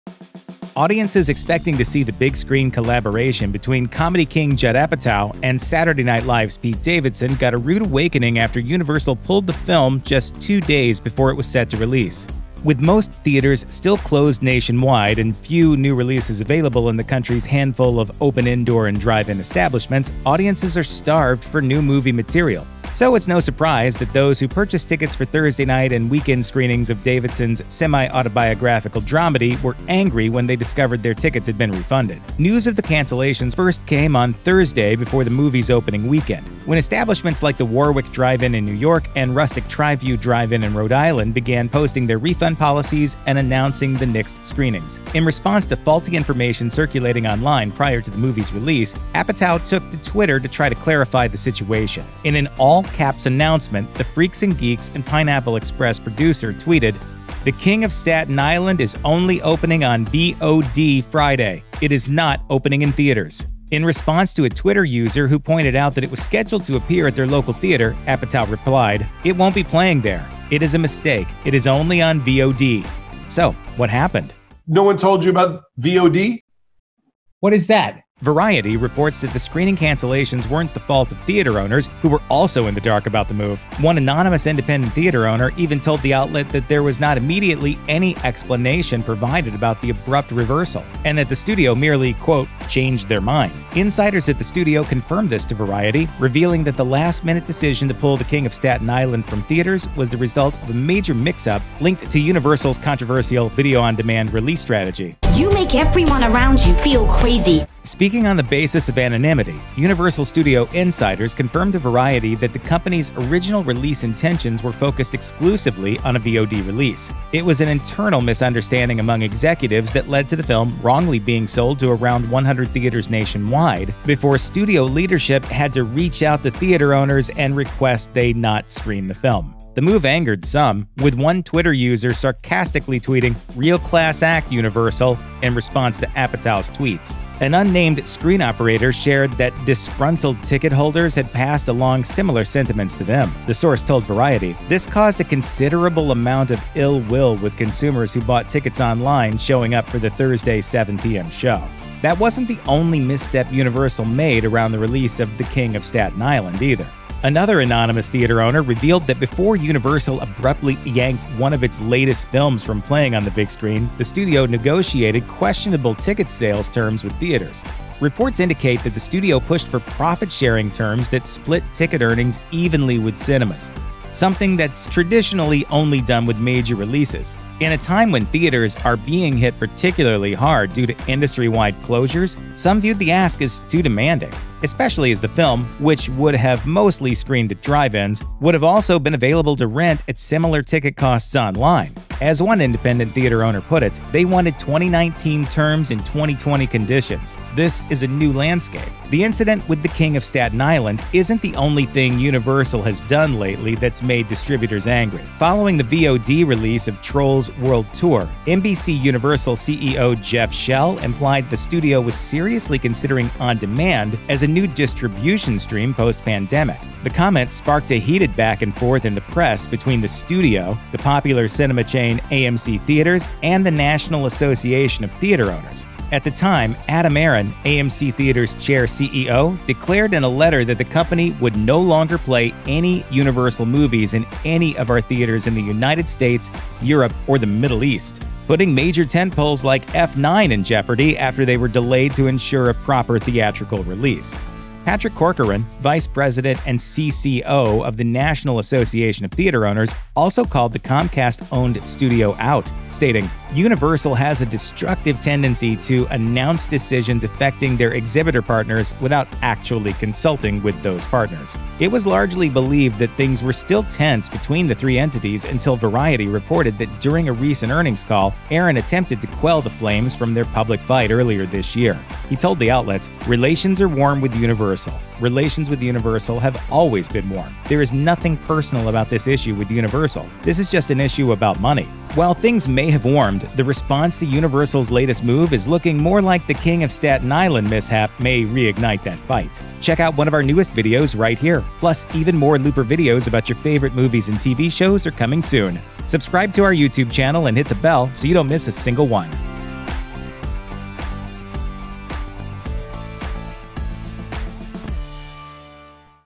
The Real Reason Pete Davidson's The King Of Staten Island Was Pulled From Theaters
Pete Davidson is one of SNL’s most popular performers, and Judd Apatow has created some of the most hilarious comedic movies of the last 20 years, so comedy fans were ecstatic when they heard that the two were collaborating on the comedy-drama The King of Staten Island.

Sadly, that excitement has halted, because the long-awaited movie has been pulled two days before its scheduled release date. This naturally angered fans who had already bought tickets and were starving for mindless laughs.

So, why was the movie pulled? Here’s the real reason Pete Davidson’s The King of Staten Island was pulled from theaters.